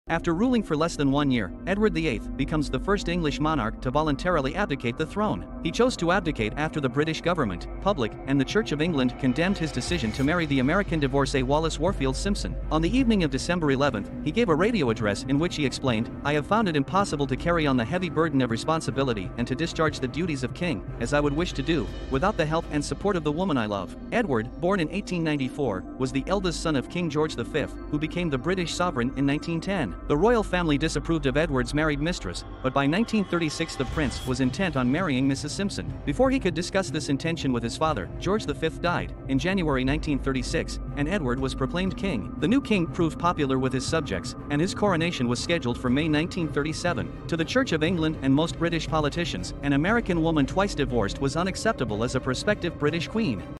Why Did England's Monarch Edward VIII Abdicate The Throne? Love Vs King
After ruling for less than one year, Edward VIII becomes the first English monarch to voluntarily abdicate the throne.

He chose to abdicate after the British government, public, and the Church of England condemned his decision to marry the American divorcée Wallis Warfield Simpson.